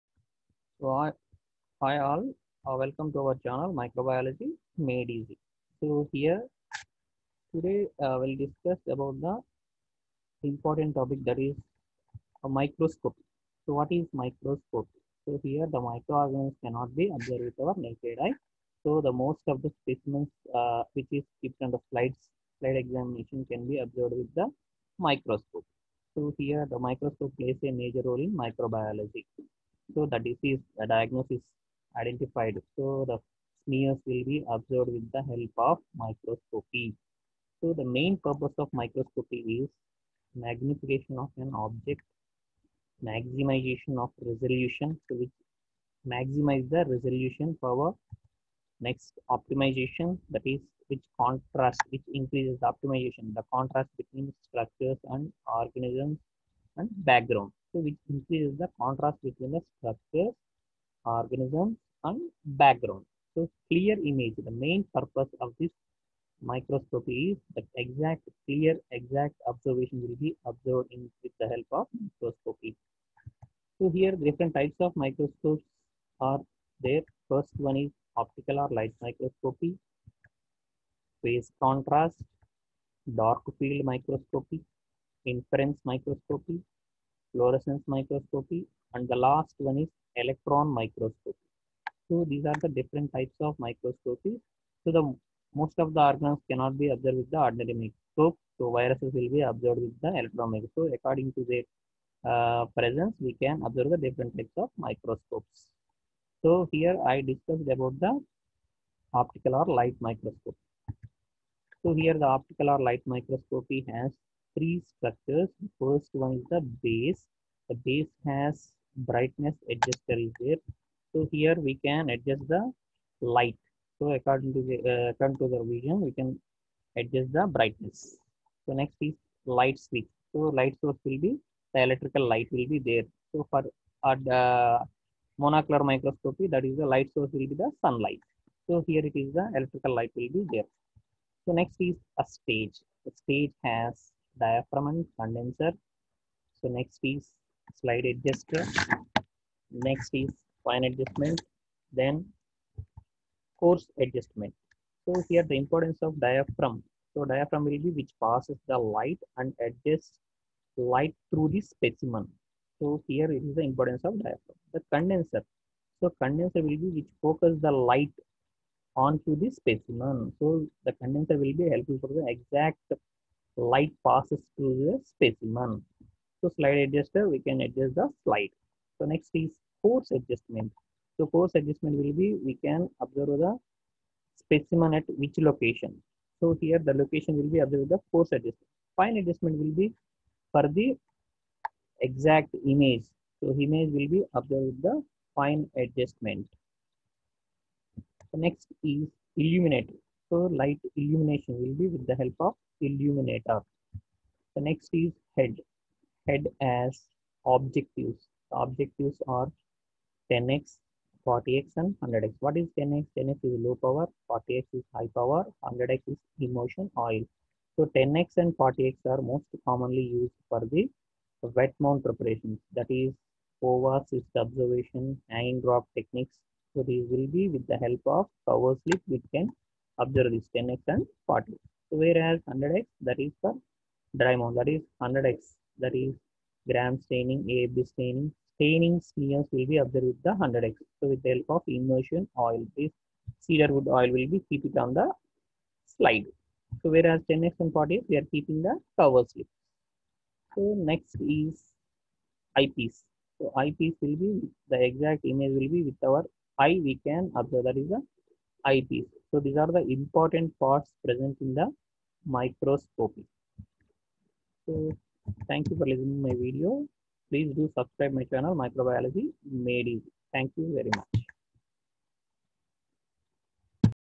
PARTS OF OPTICAL/LIGHT MICROSCOPY
parts of microscopy
Purpose
Magnification of object
Maximisation of resolution
Optimization

contrast between structures, organisms & background
parts
1.base
2.stage
3.head